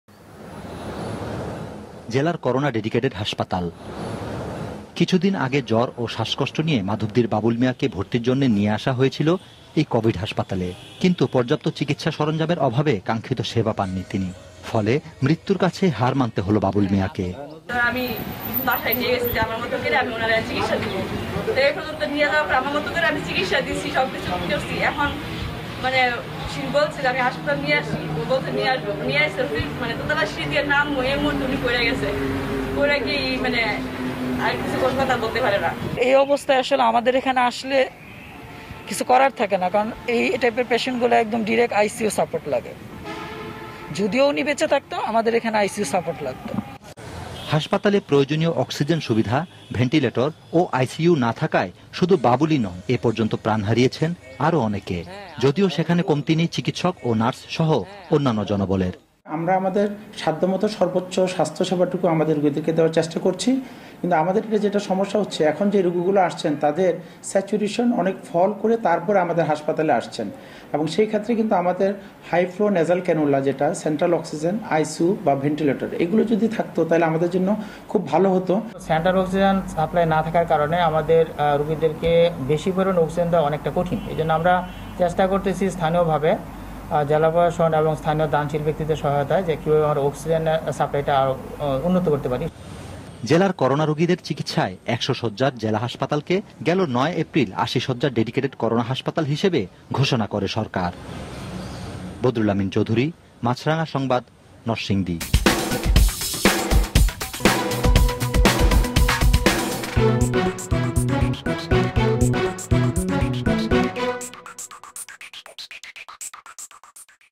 করোনা আক্রান্ত মুমূর্ষু রোগিদের চিকিৎসা দেয়া যাচ্ছে না নরসিংদীর জেলা কোভিড হাসপাতালে
পর্যাপ্ত চিকিৎসক ও নার্স থাকলেও আইসিইউ, প্রয়োজনীয় অক্সিজেন সুবিধা ও ভেন্টিলেটর না থাকায়, করোনা আক্রান্ত মুমূর্ষু রোগিদের চিকিৎসা দেয়া যাচ্ছে না নরসিংদীর জেলা কোভিড হাসপাতালে। প্রায়ই মারা যাচ্ছে রোগী।